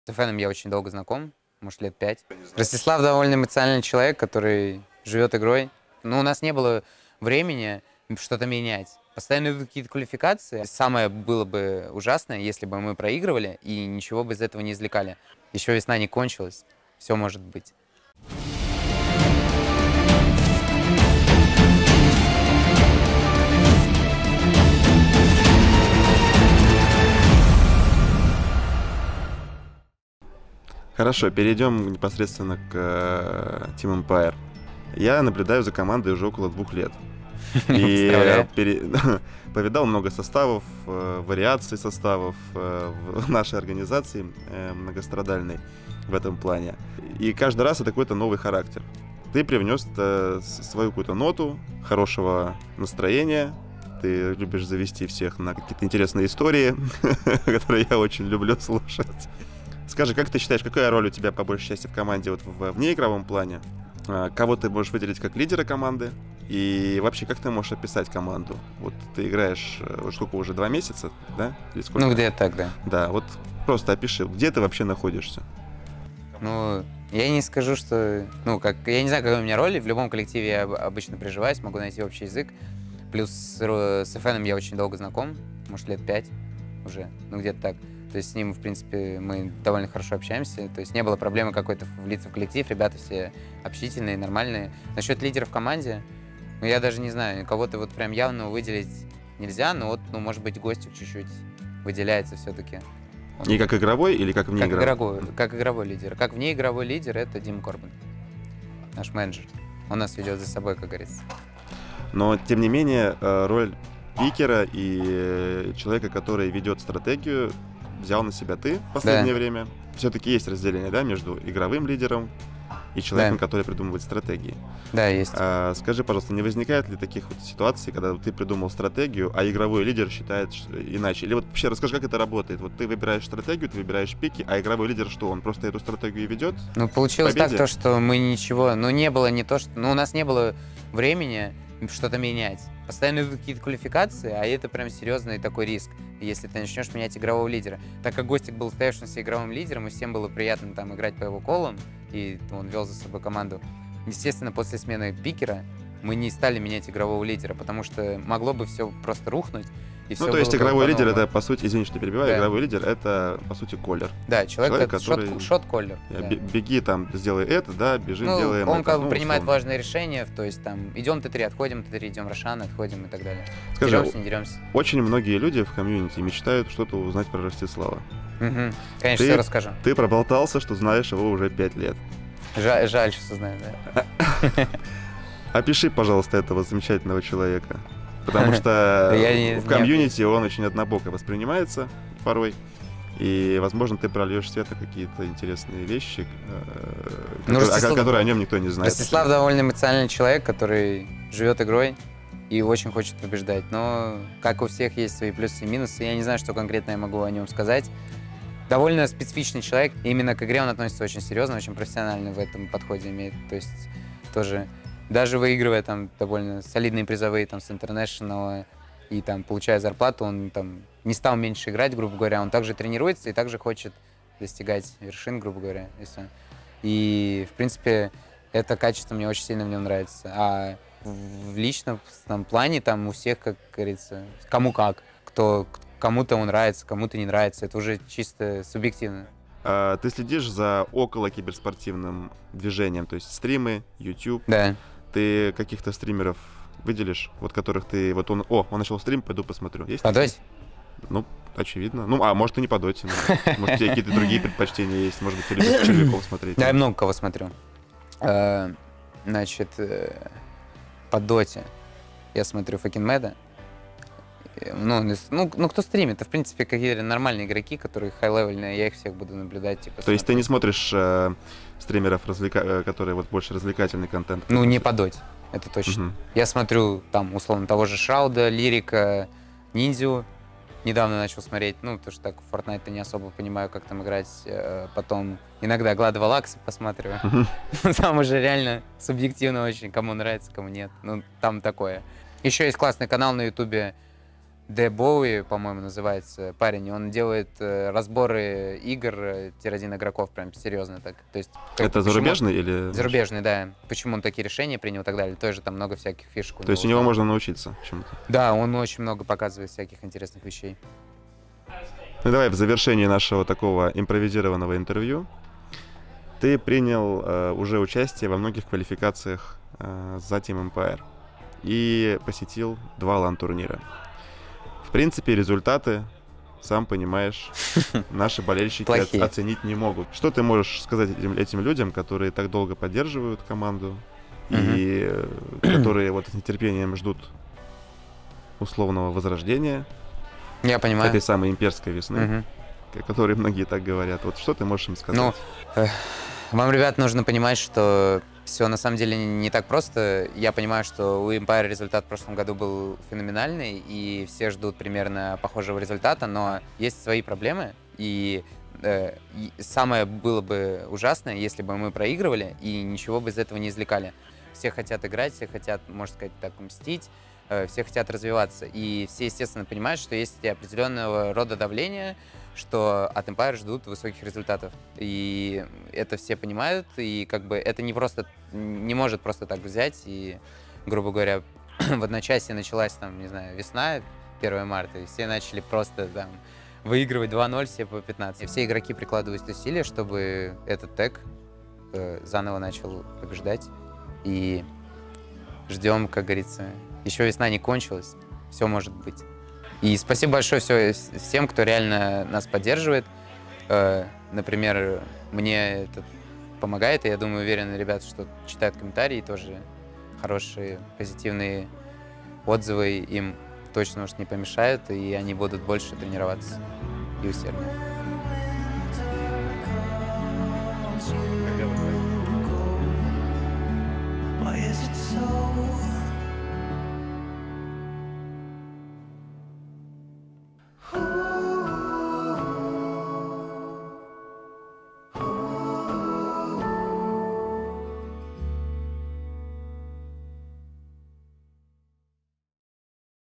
Интервью с Nix: Часть 2 (об игроках Team Empire, об FN!'е, о стримерах) | Empire on Lan @ Dota 2 Pro
Вторая часть Интервью c Nix из Team Empire на DreamLeague Season 9
о команде, о fn'e, о стримерах, обращение к болельщикам.